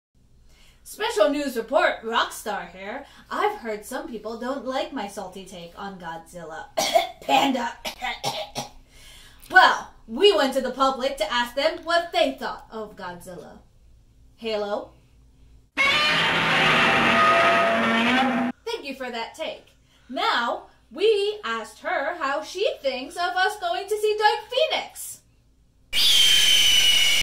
Breaking News: Response to Reviews
Roxstar was unimpressed in our last movie review, but what does the audience have to say about her take and another upcoming review of ours?